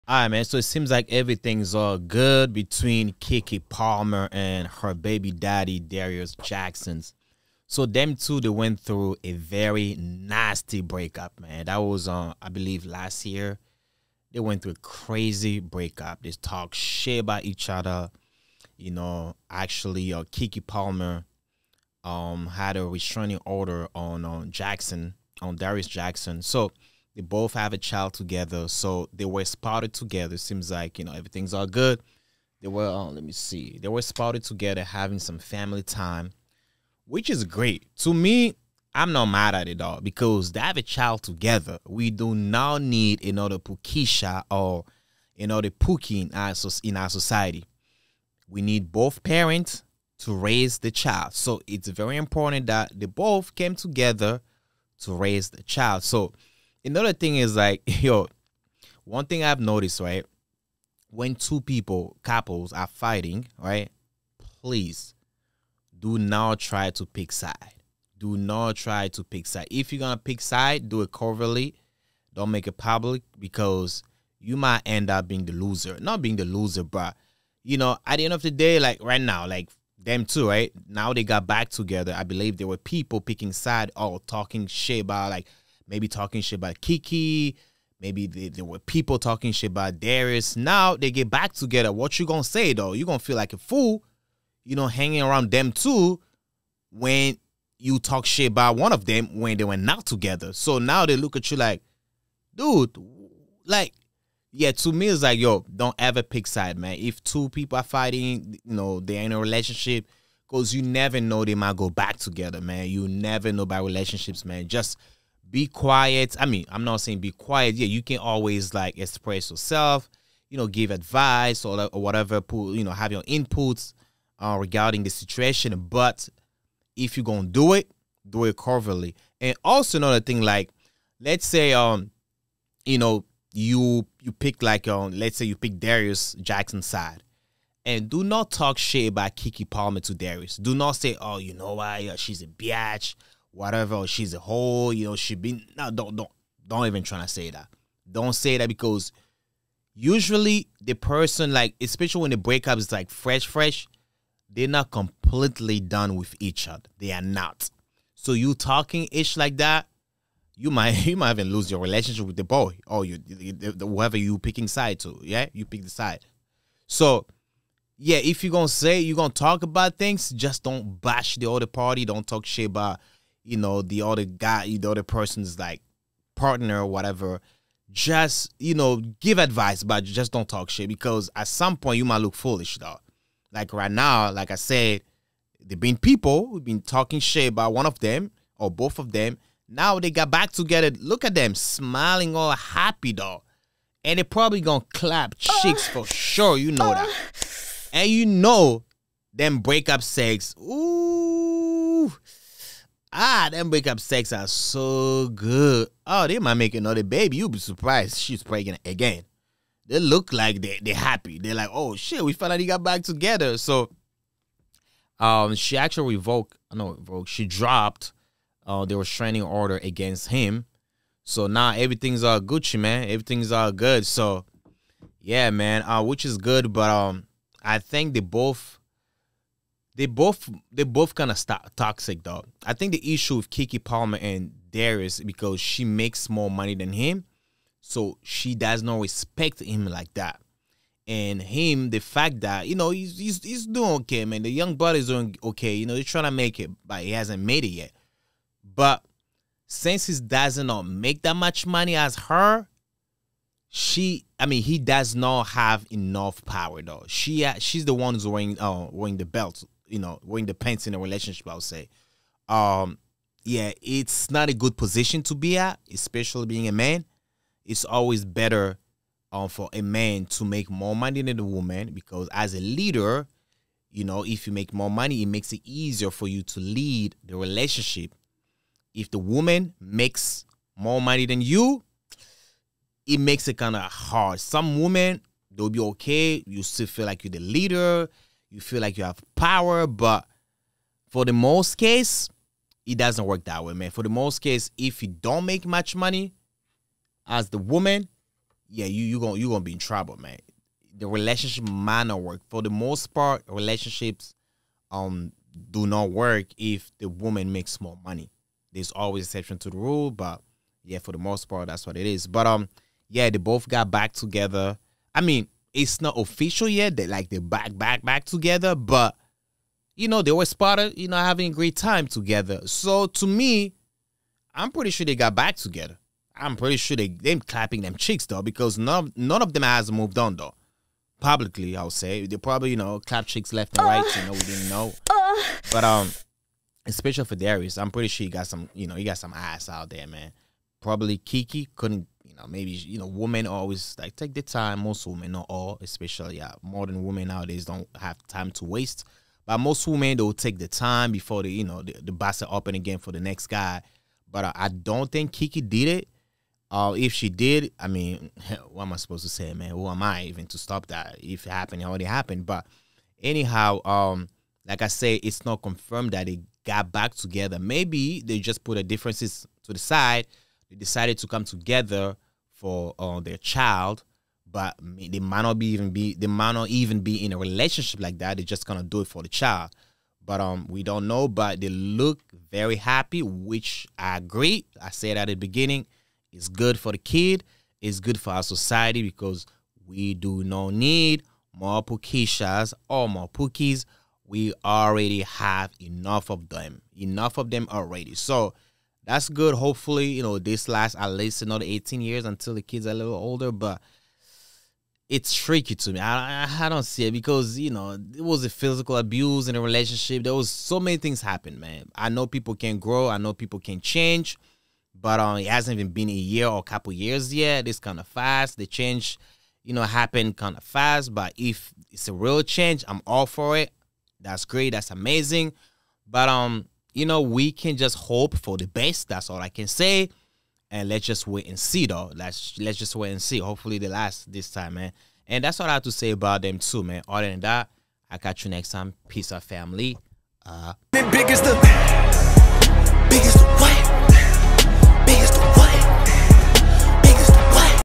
Keke Palmer is finally back with her baby daddy, possibly with baby number 2 on the way.
Thank you for your viewership! This channel is committed to covering popular topics, celebrity news, documentaries, and reaction videos.